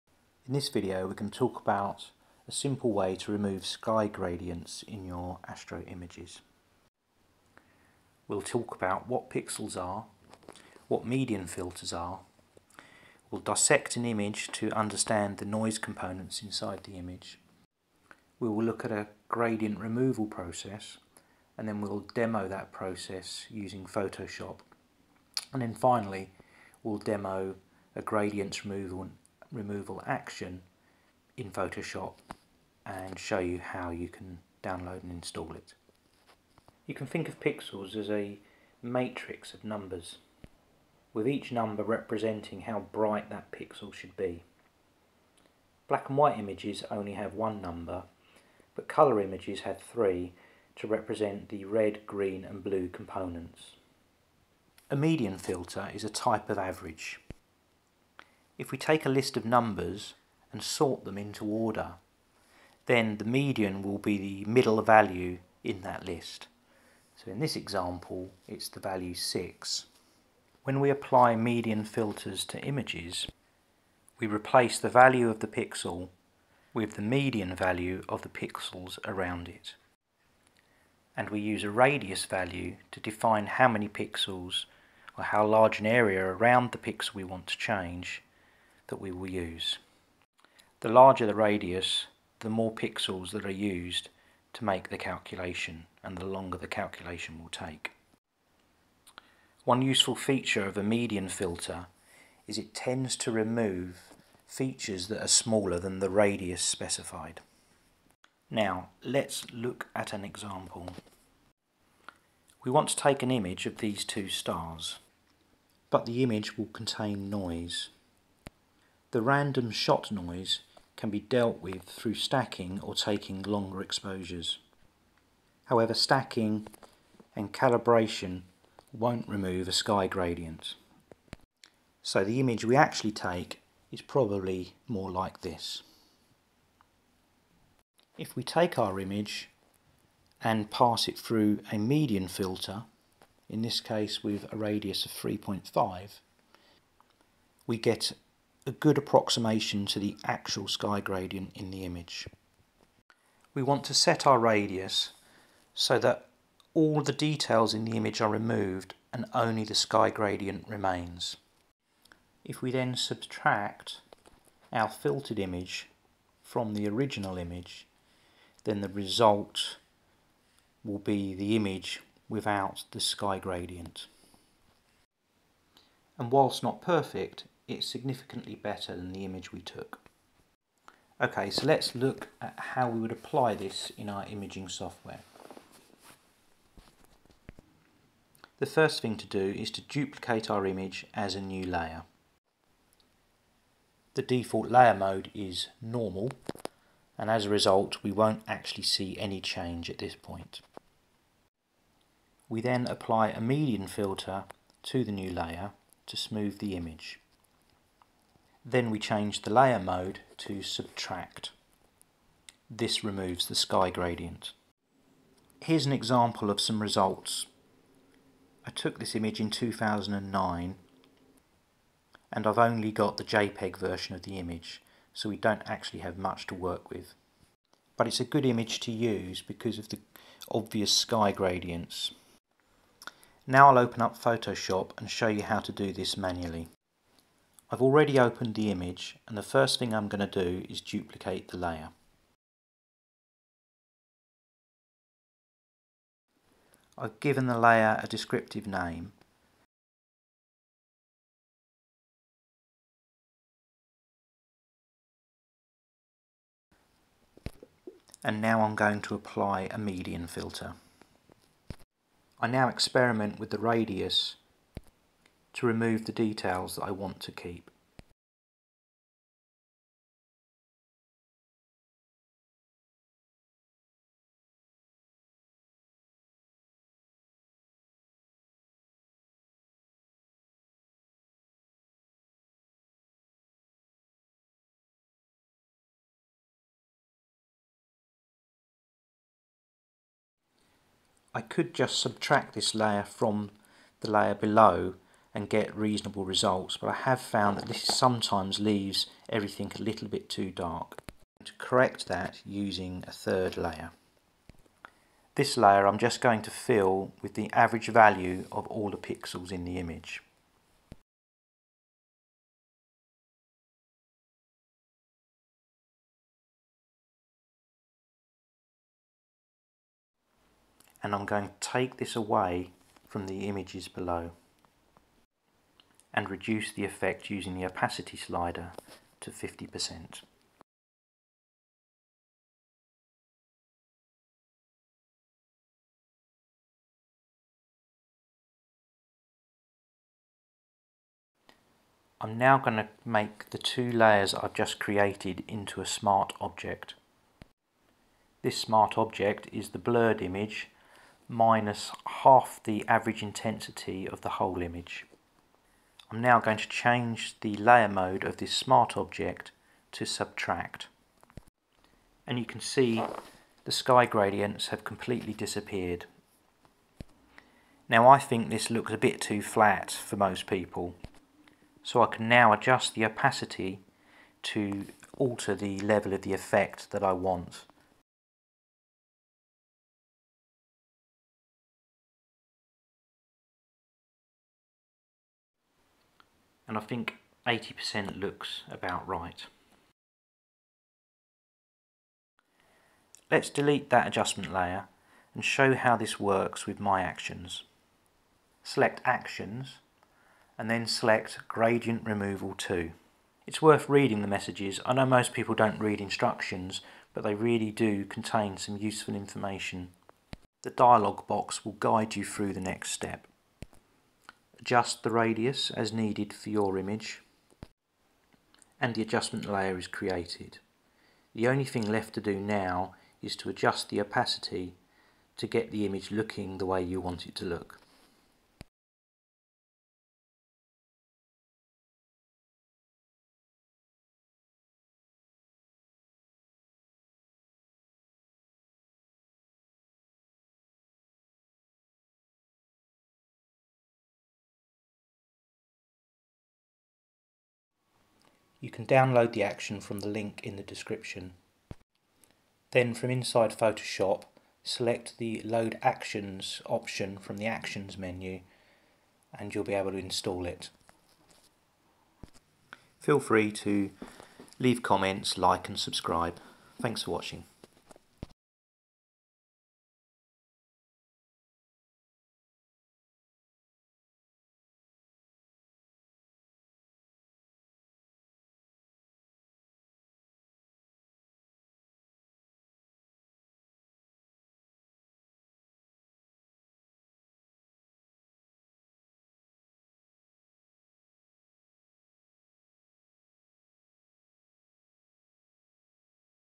Free Photoshop Action to fix Gradients in Astro Images
Got a particularly difficult astro-image with nasty gradients?
Find out how to remove them with this process and free action for photoshop.

In this video I take you through my process for fixing sky gradients in astrophotography images. I then demo the approach in photoshop then demo the same approach using my Free Astro Actions.

You can download SimonTelescopium's Astro Actions [STAA] from here